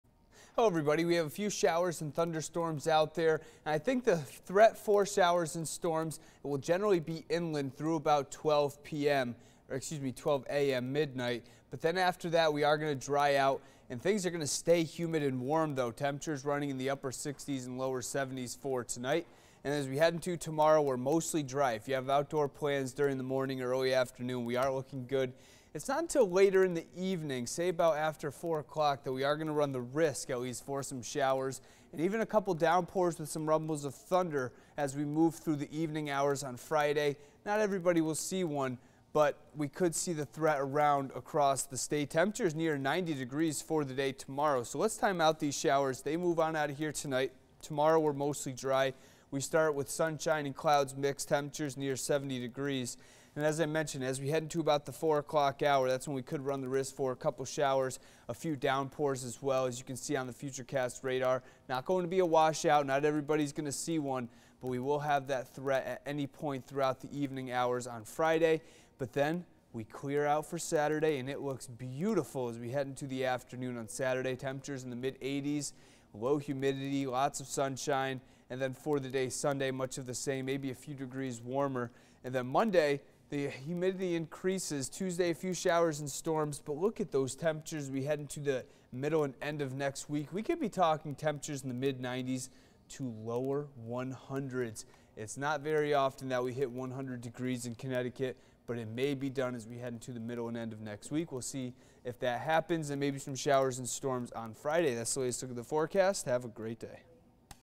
Thursday Evening Weather Update
Mostly dry Friday with some evening showers and storms possible